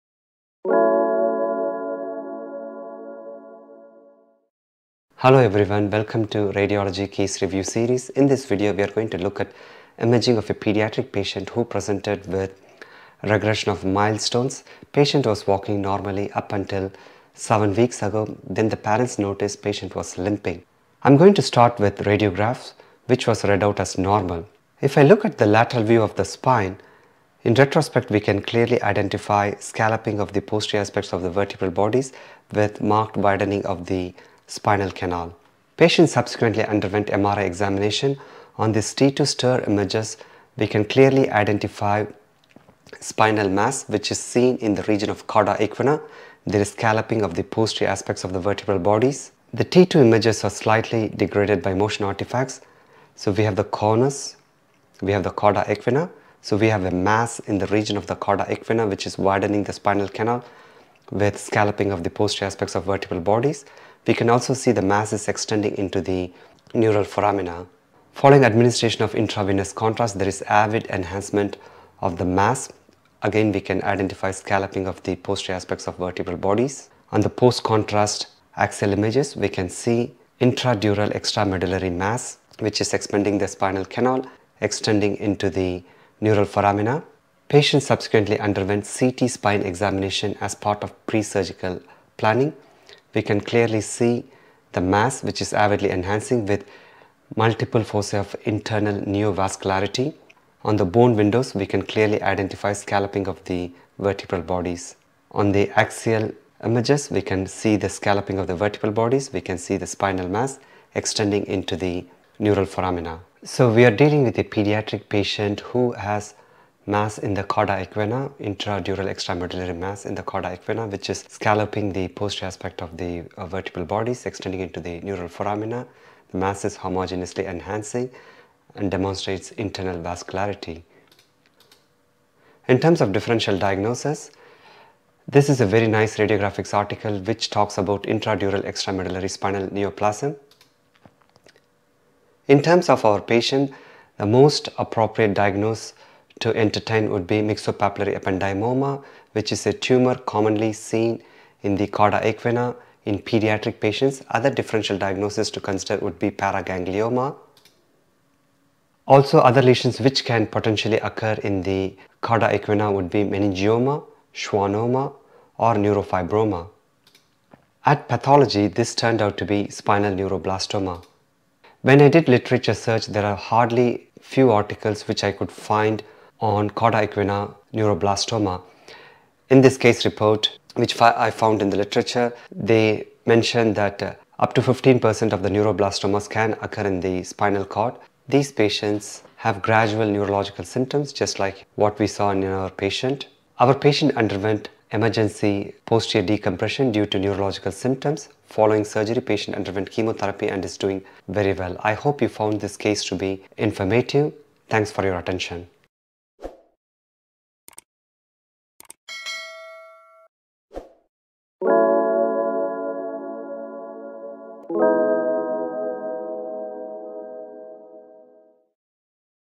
Spinal Neuroblastoma
Speaker: Dr. Balaji Rao, MBBCh
Assistant Professor, Emergency Radiology and Neuroradiology,
Dept of Radiology and Biomedical Imaging,